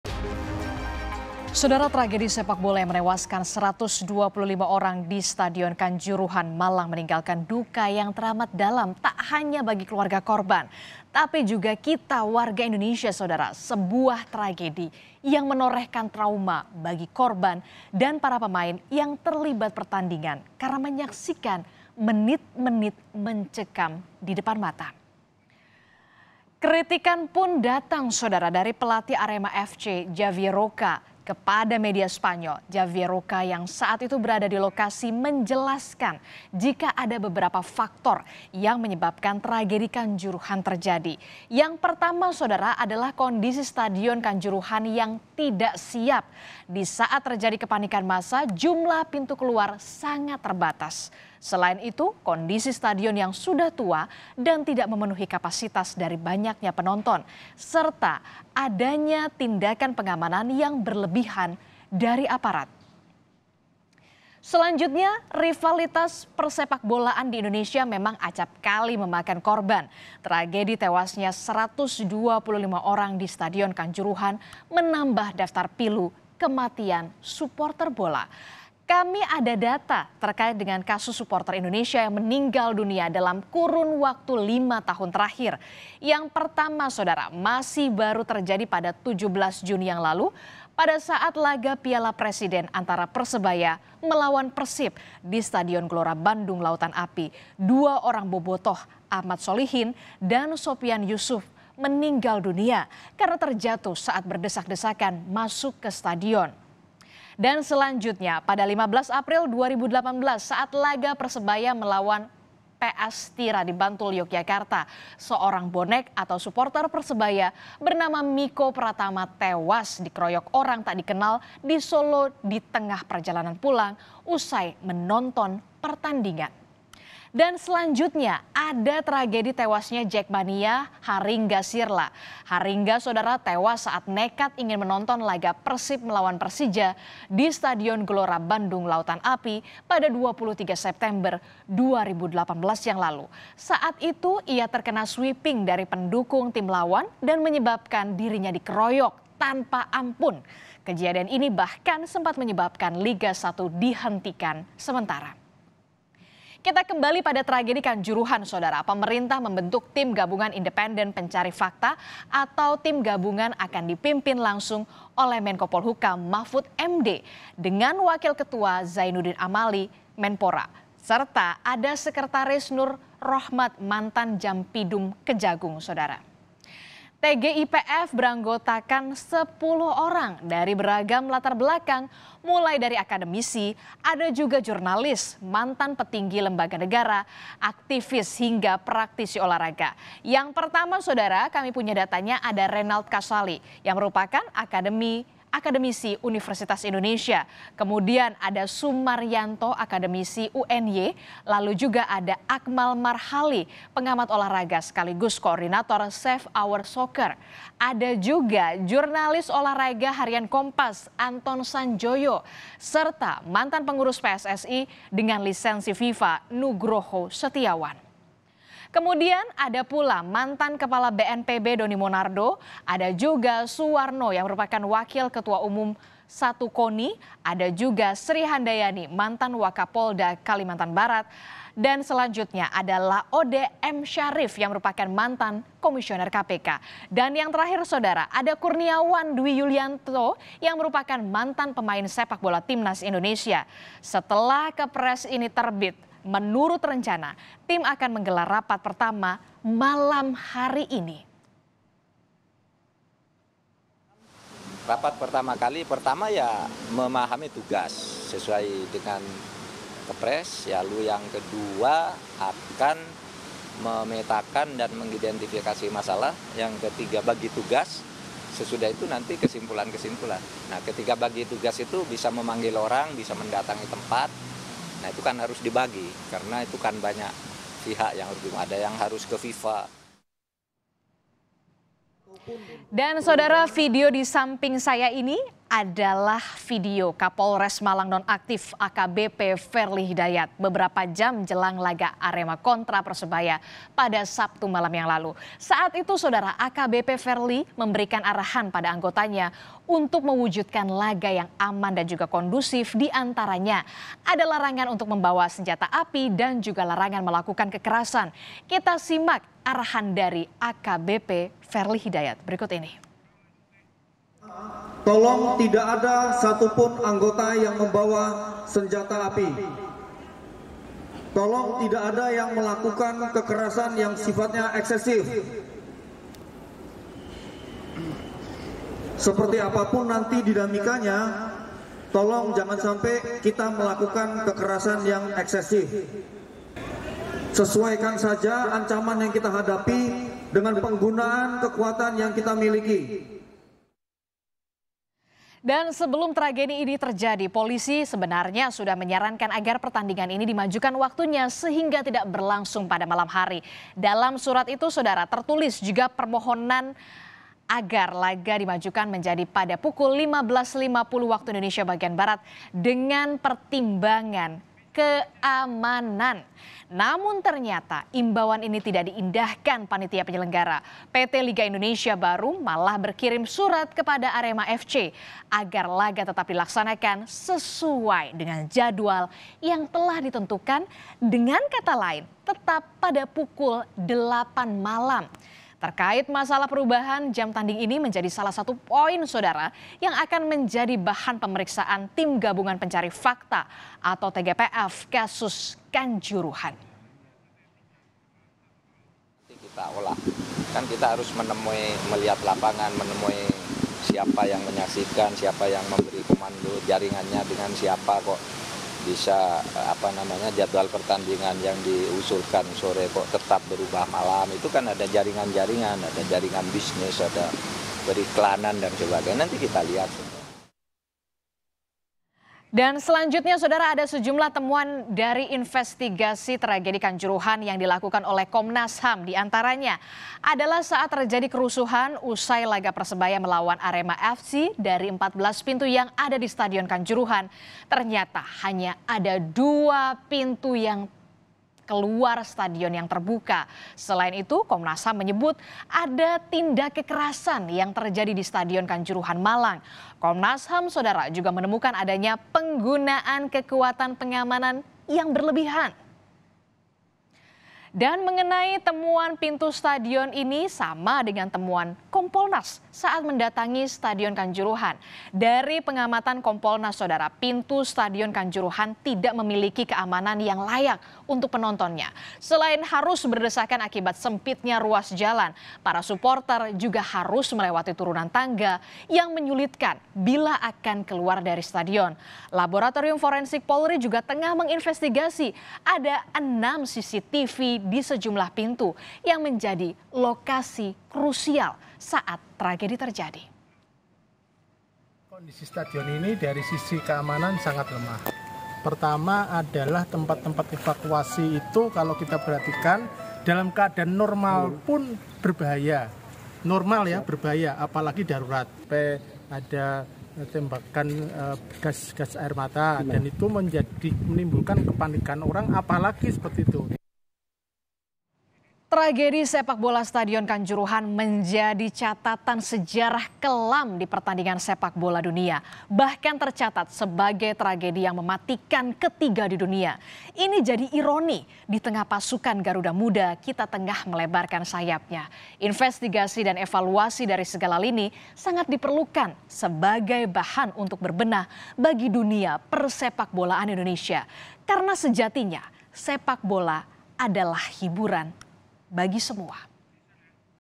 Tragedi Kanjuruhan Jadi Catatan Kelam Sejarah Pertandingan Sepak Bola Dunia
KOMPAS.TV - Tragedi sepak bola yang menewaskan 125 orang di Stadion Kanjuruhan, Malang meninggalkan duka yang teramat dalam.

Tak hanya bagi keluarga korban, tapi juga kita warga Indonesia.

Baca Juga Pelatih Manchester City dan MU Beri Komentar Soal Tragedi Kanjuruhan di

Sebuah tragedi yang menorehkan trauma bagi korban dan para pemain yang terlibat pertandingan karena menyaksikan menit-menit mencekam di depan mata.

Kritikan pun datang dari pelatih Arema FC Javier Roca. Kepada media spanyol Javier Roca yang saat itu berada di lokasi menjelaskan jika ada beberapa faktor yang menyebabkan tragedi kanjuruhan terjadi.

Pertama adalah kondisi stadion kanjuruhan yang tidak siap di saat terjadi kepanikan massa jumlah pintu keluar sangat terbatas.

Selain itu kondisi stadion yang sudah tua dan tidak memenuhi kapasitas dari banyaknya penonton. Serta adanya tindakan pengamanan yang berlebihan dari aparat.

Rivalitas persepakbolaan di Indonesia memang acap kali memakan korban. Kematian ratusan di Kanjuruhan menambah daftar kematian supporter bola.

Tragedi sepak bola stadion Kanjuruhan menjadi catatan sejarah kelam di pertandingan sepak bola dunia.

Bahkan tercatat sebagai tragedi mematikan ketiga di dunia. Ini jadi ironi di tengah pasukan garuda muda kita tengah melebarkan sayapnya.

Investigasi dan evaluasi dari segala lini sangat diperlukan sebagai bahan untuk berbenah bagi dunia persepakbolaan Indonesia. Karena sejatinya sepak bola adala hiburan bagi semua.